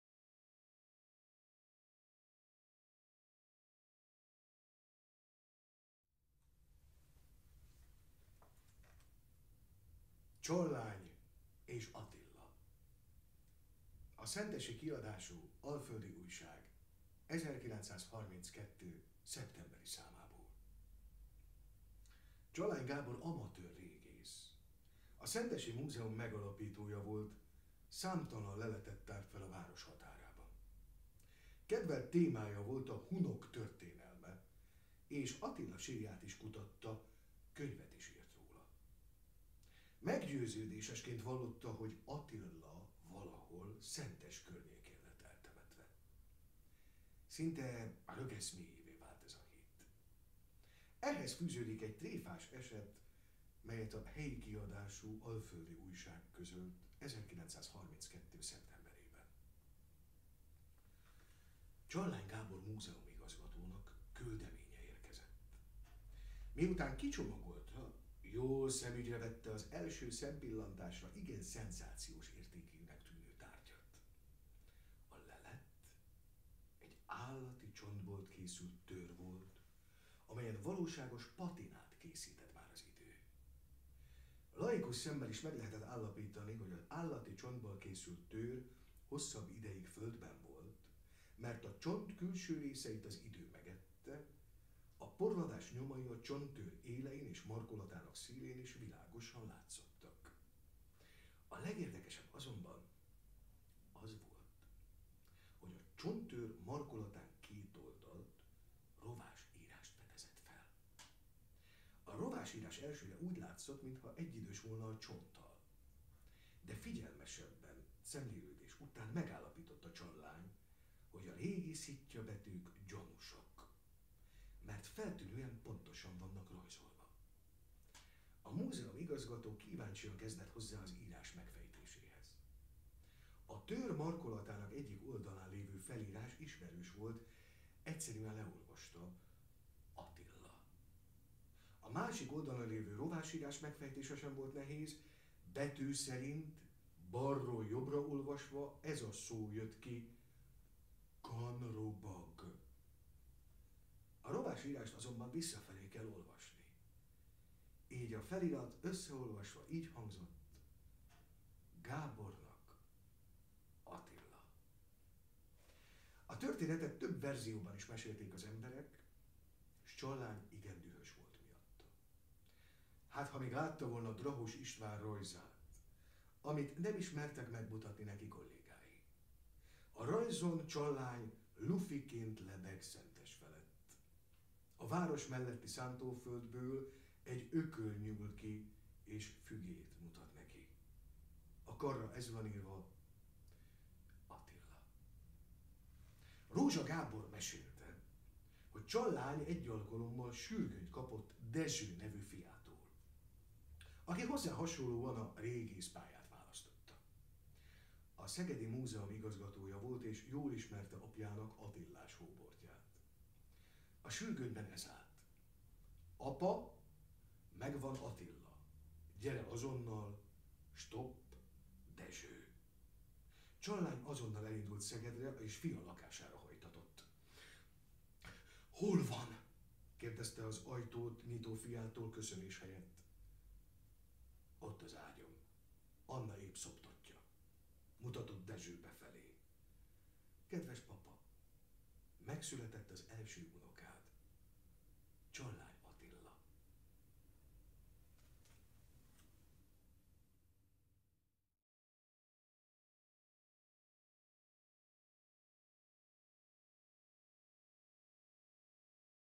Csallány és Attila - Szentesi anekdoták, Első rész
"Formabontó megoldással jelentkezik a Koszta József Múzeum, a járványhelyzethez igazodva az interneten keresztül folytatja muzeológiai tevékenységét együttműködésben a városi könyvtárral. Tízrészes helytörténeti sorozatot készítenek Vágvölgyi Zoltán Szentesi anekdoták című kötetéből, Farkas László Róbert múzeumigazgató tolmácsolásában."
Forrás: szentesielet.hu